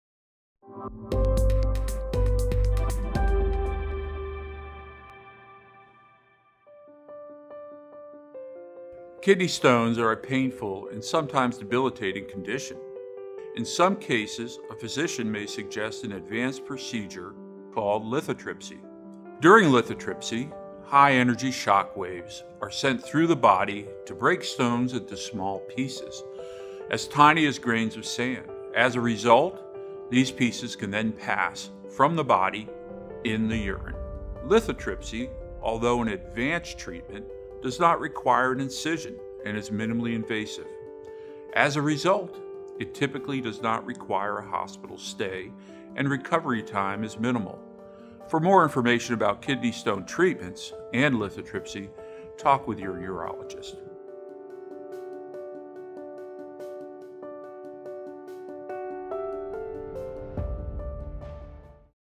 The Urology Group: Lithotripsy for Kidney Stones
Lithotripsy is often used to treat kidney stones that are too large to pass on their own. The procedure allows the patient to lay in a comfortable position while the machine rotates around the affected area. Dr. Christopher Cirulli explains more.